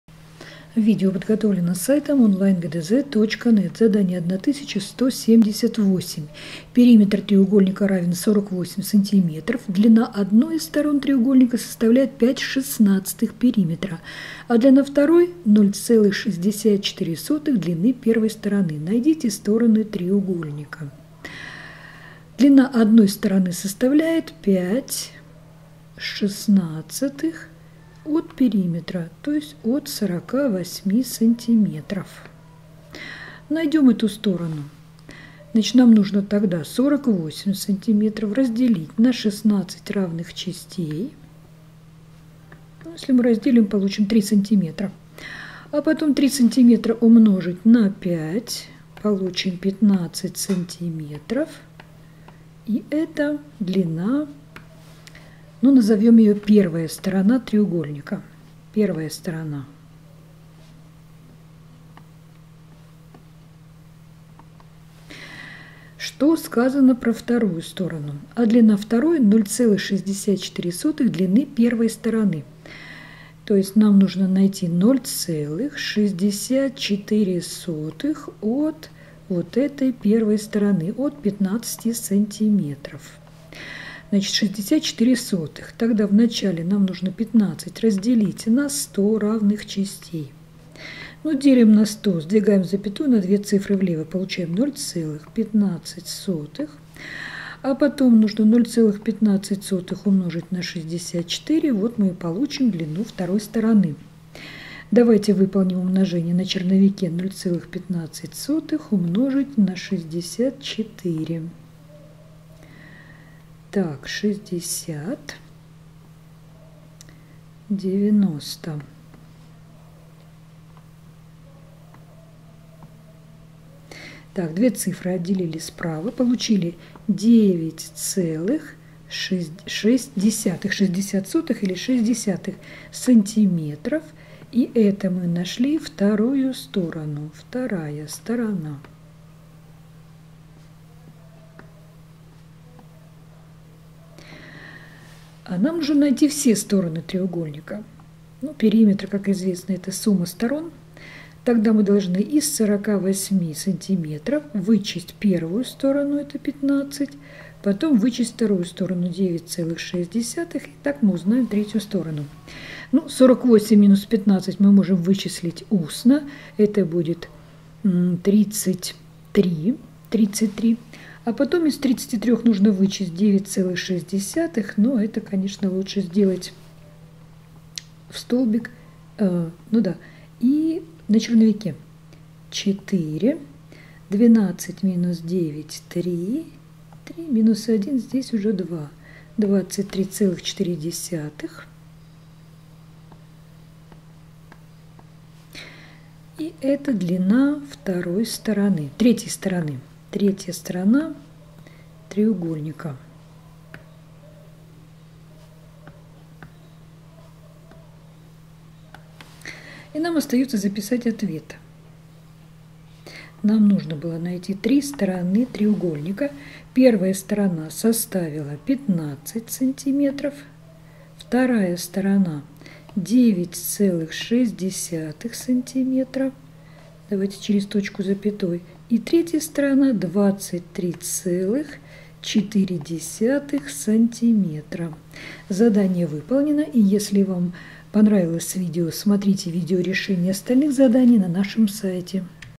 Задание №1178 - Математика 5 класс (Мерзляк А.Г., Полонский В.Б., Якир М.С)
Периметр треугольника равен сумме его сторон ( см. 14-й параграф). Поэтому, вычислив длины первой и второй стороны, можно найти длину третьей стороны треугольника. Чтобы сделать это задание без ошибок, воспользуйтесь нашим видео с решением.